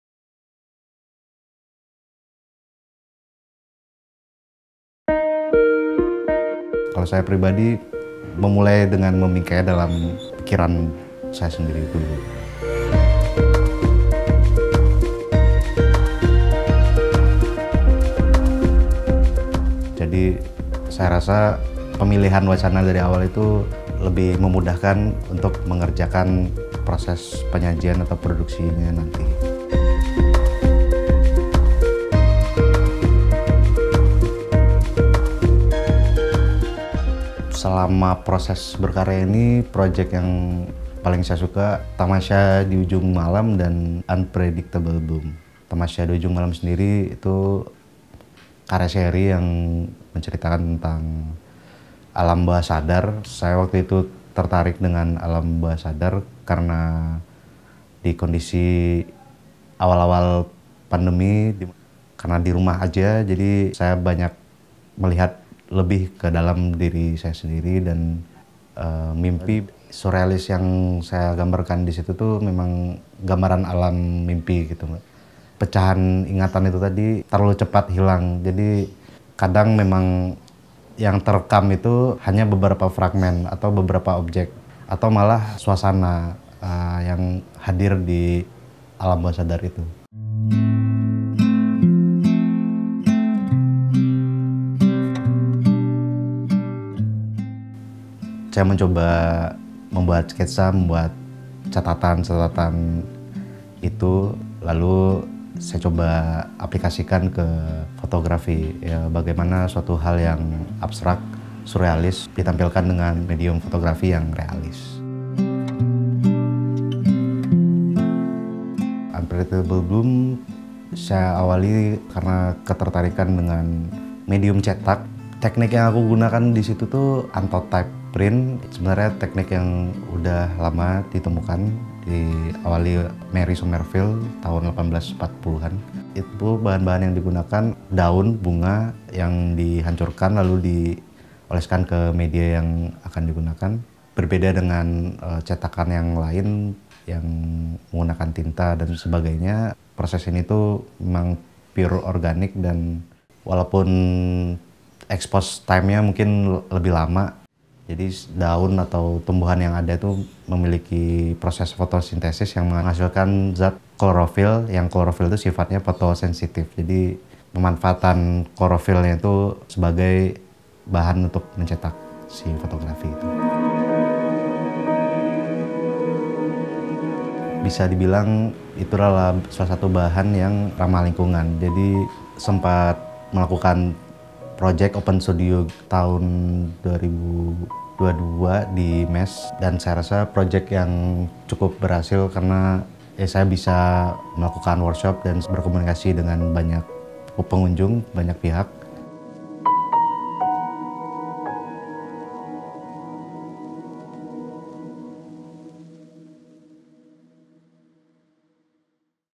M ALFARIZ
M Alfariz is a contemporary artist based in Yogyakarta. His work primarily focuses on natural environments, suggesting a commentary on the relationship between humanity and the natural world. See what inspires him behind his most recent artworks in our documentary.

Supported by Srisasanti Syndicate

M Alfariz adalah seniman kontemporer yang berbasis di Yogyakarta. Karyanya banyak berfokus pada lingkungan dan alam sekitar, menyiratkan tentang hubungan antara manusia dan dunia di sekitarnya. Lihat apa yang menginspirasinya di balik karya-karya terbarunya dalam dokumenter kami.

Didukung oleh Srisasanti Syndicate